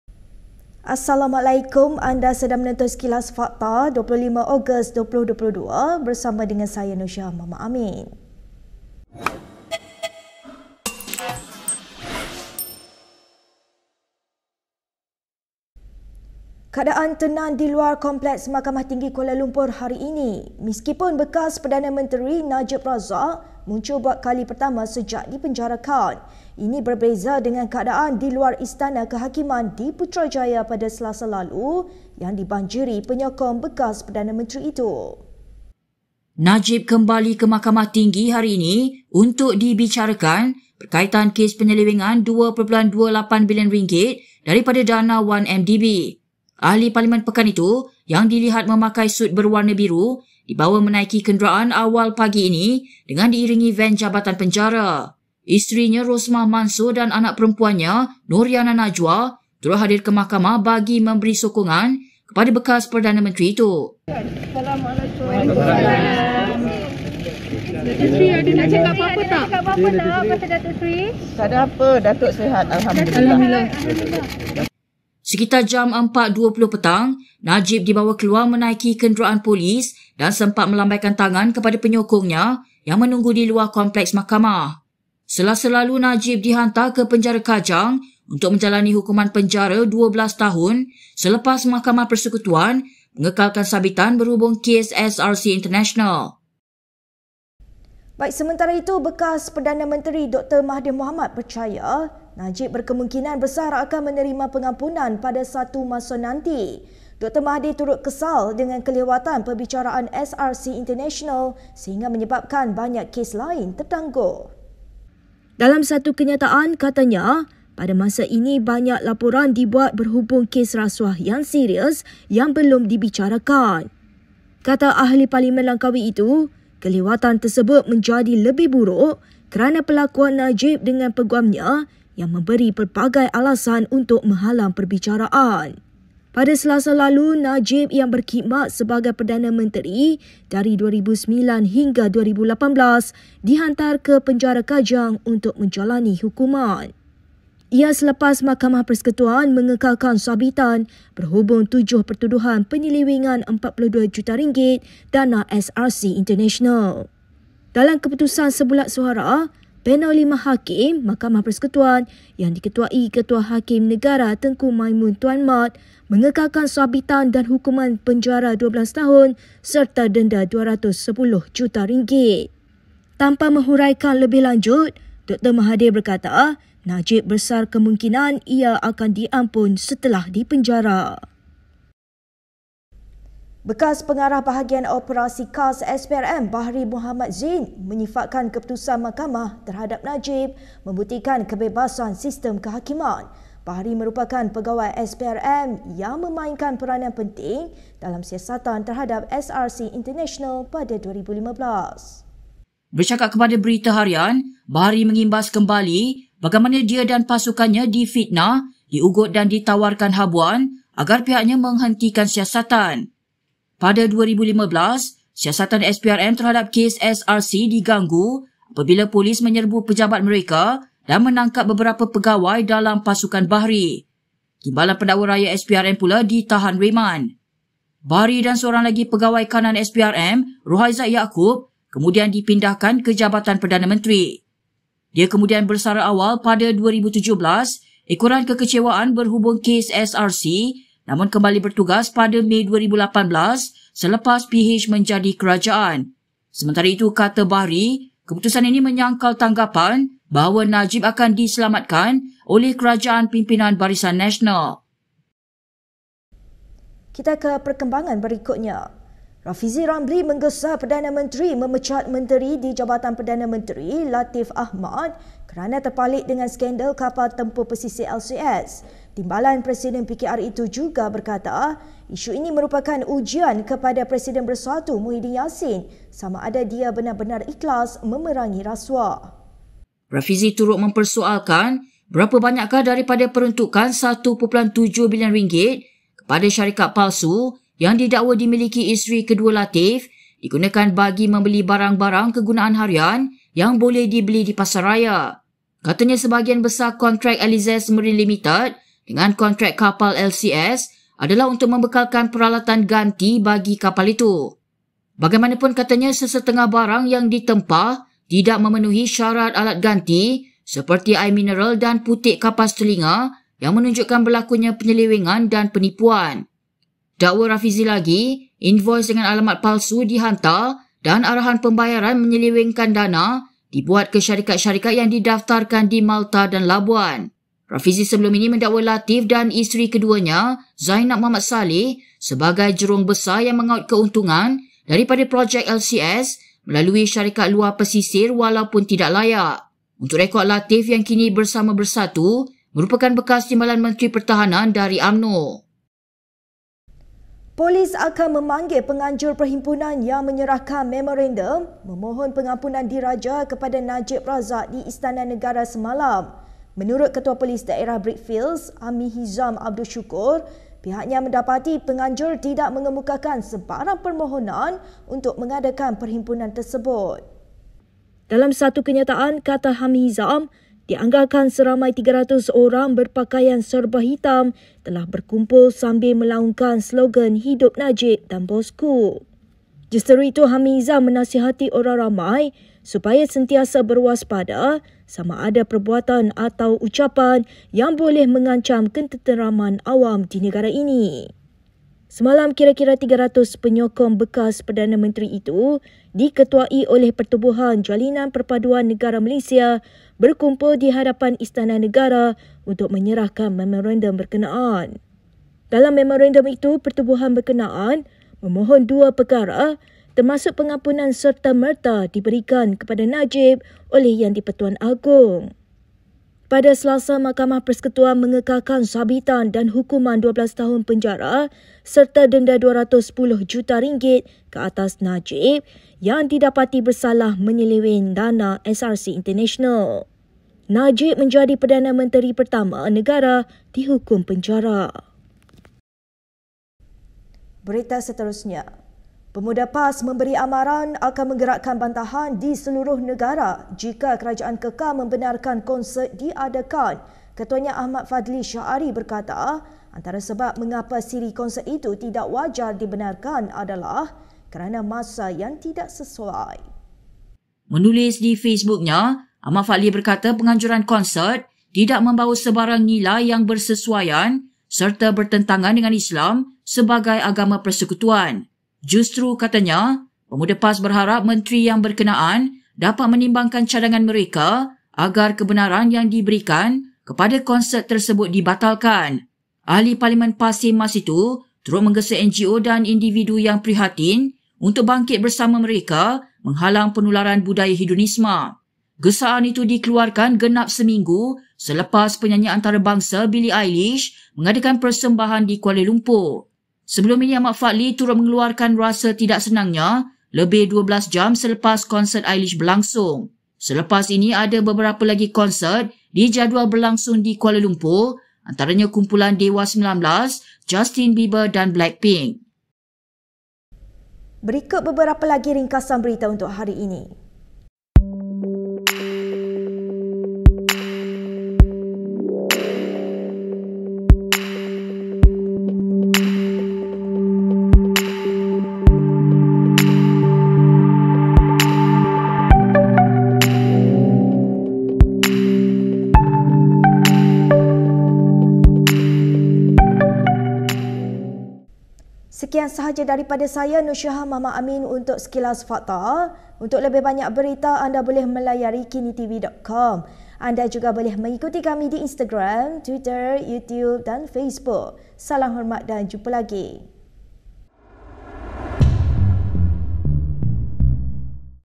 Najib muncul di mahkamah, Najib mungkin diberi pengampunan, PM perlu pecat Latiff! |SEKILAS FAKTA
Antara paparan Sekilas Fakta 25 Ogos 2022

1) Najib muncul di mahkamah dari Penjara Kajang untuk kes 1MDB
2) Besar kemungkinan Najib akan diberi pengampunan - Dr Mahathir
3) Bekas pengarah SPRM tak lupa fitnah, ugutan ketika siasat kes SRC
4) Terpalit skandal LCS, Rafizi gesa PM pecat Latiff dari kabinet
5) Penganjur himpunan mohon pengampunan Agong untuk Najib disiasat
6) 'PAS tak teragak-agak bantah seluruh negara jika konsert diteruskan'